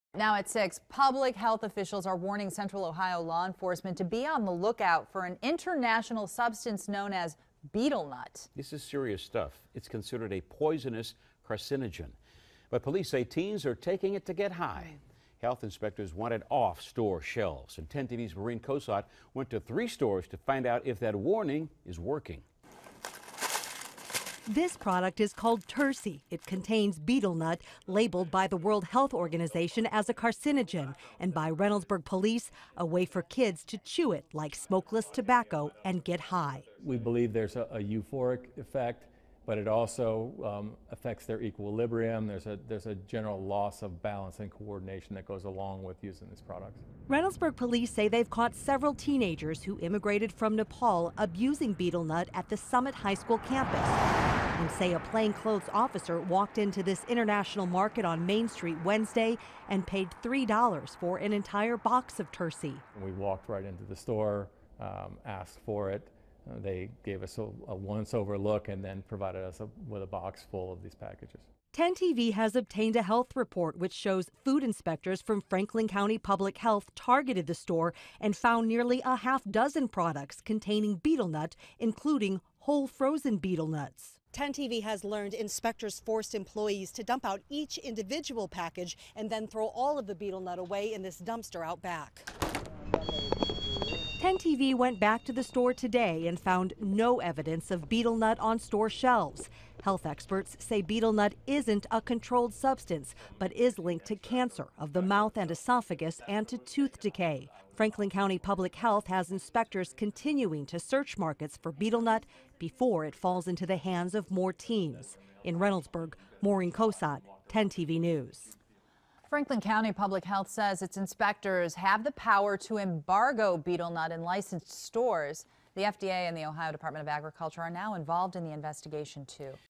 Betel nut cancer
Tulsi carcinogenic banned. Don't chew betel nuts